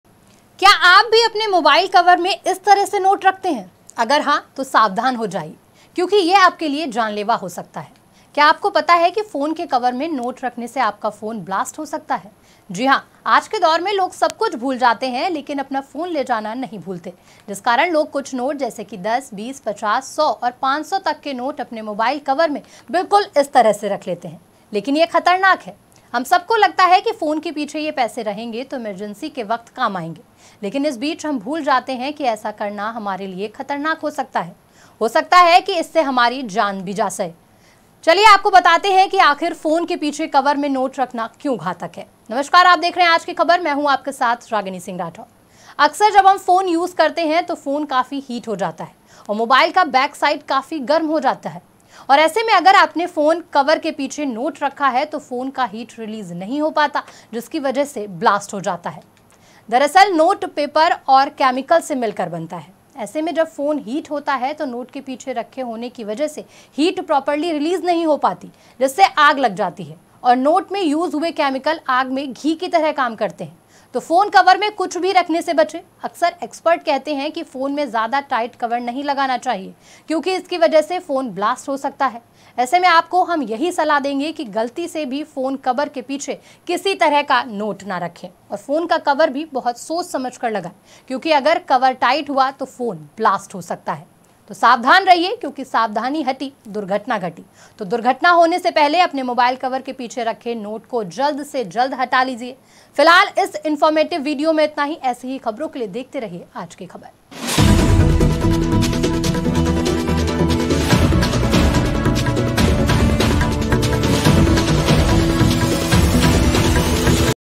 क्या आप भी रखते है मोबाइल कवर में नोट ? || Aaj ki khabar
नोट में मौजूद केमिकल की वजह से ये आग और भी बढ़ सकती है. ऐसे में आपको हम यही सलाह देंगे कि गलती से भी फोन कवर के पीछे किसी तरह का नोट ना रखे. और फोन का कवर भी बहुत सोच समझ कर लगाएं, क्योंकि अगर कवर टाइट हुआ तो फोन ब्लास्ट हो सकता है.